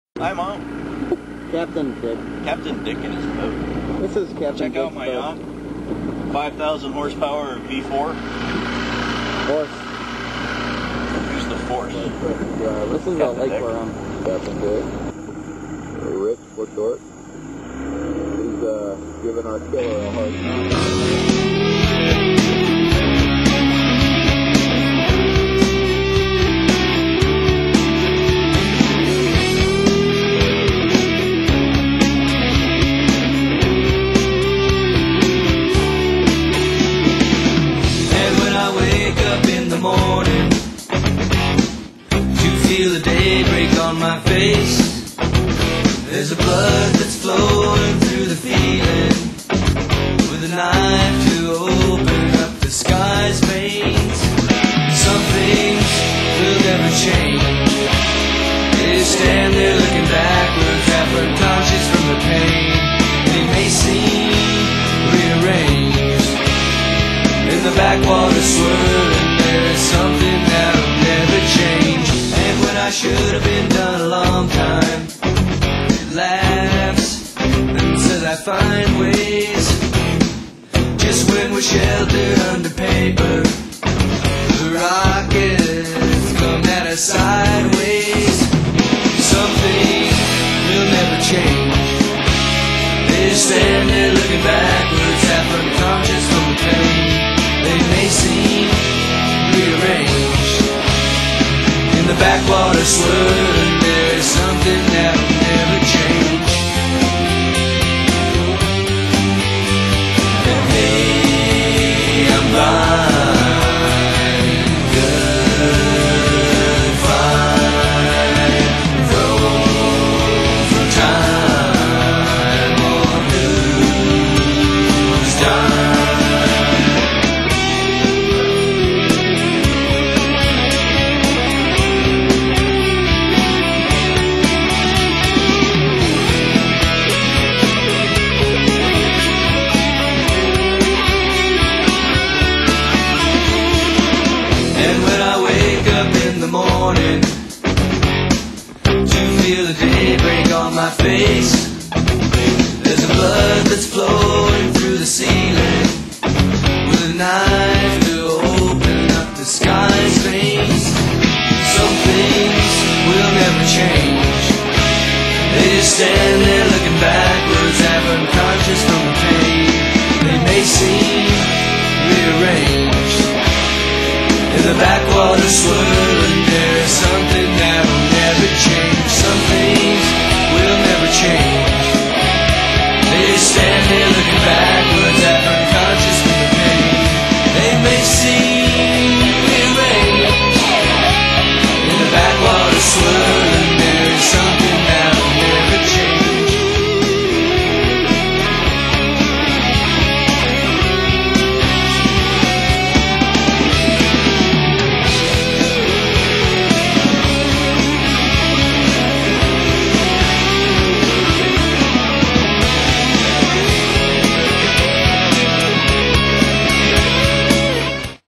Jerry Hilton with Capn' Dick water skiing in chain of lakes Winter Haven Florida 199?
Capn' Dicks Boat In Winter Haven Chain of Lakes 199?